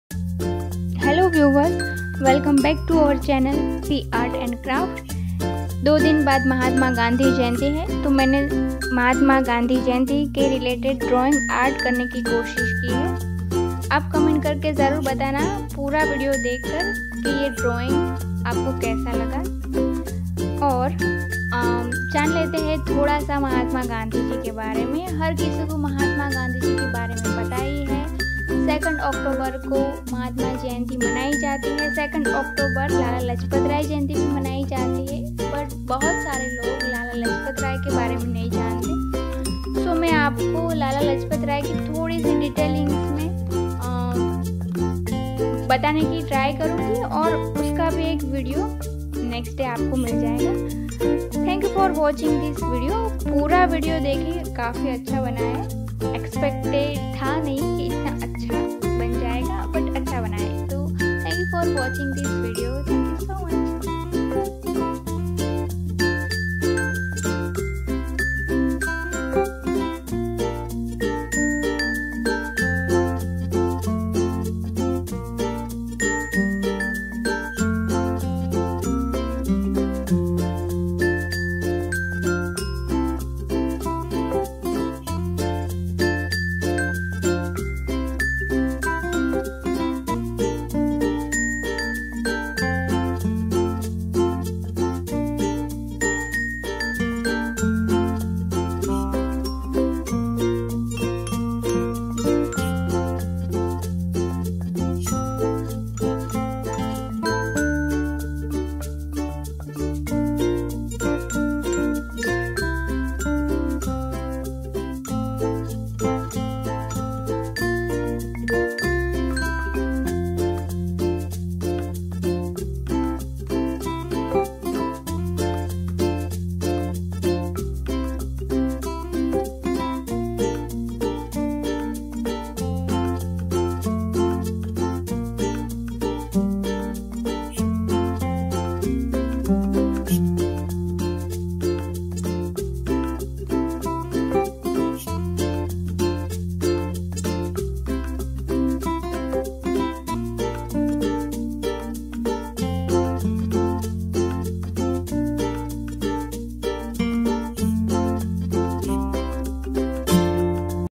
Mahatma gandhi jayenti drawing , lettering gandhi jayenti
Publish :29-9-22
Hello welcome back to our you tube channel P Art and Craft
Link:

About video:
Gandhi jayanti is celebrated annually on 2nd October to commemorate the birthday of mahatma gandhi